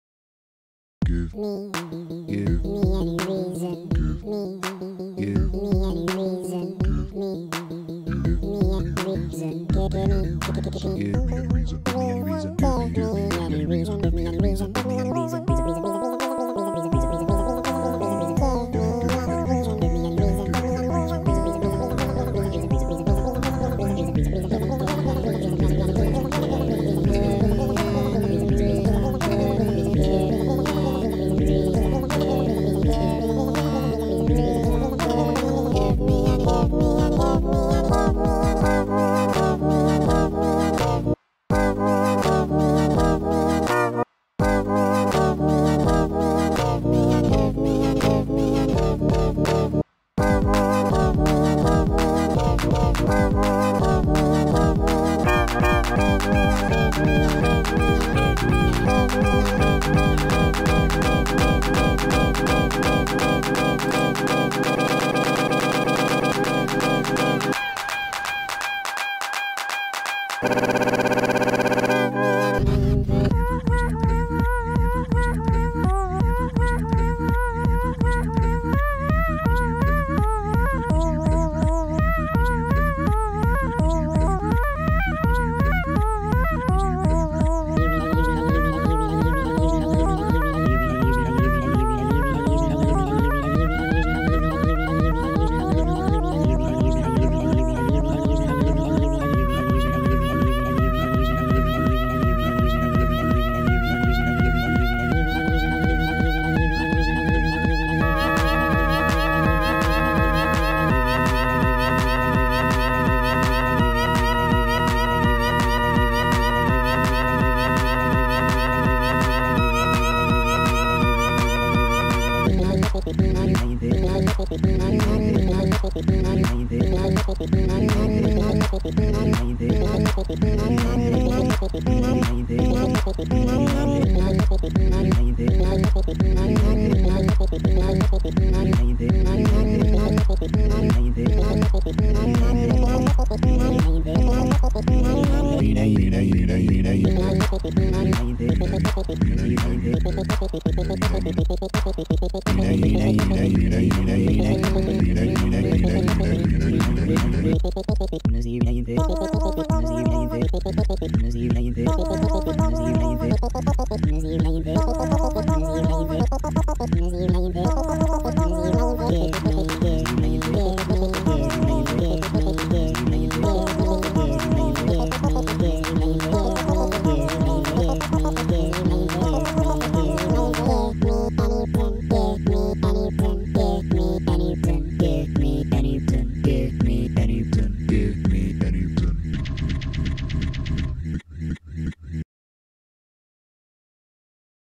gnomish music for fiendish fellows
i haven't had anything to drink in over 5 hours, making it back into the psych ward with this one woooooooo

decided to challenge myself and make a song with only one sample (except at 1:03) no automation and no effects (other than some light compression and reverb for the drums)